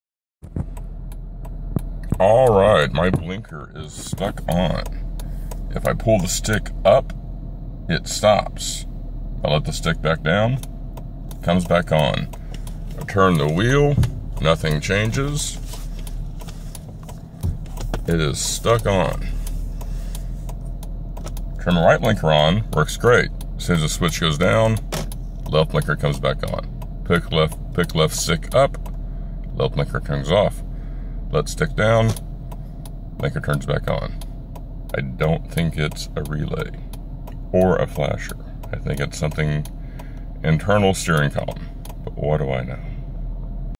Grand Cherokee stuck blinker
My 07 grand Cherokee with the left blinker stuck on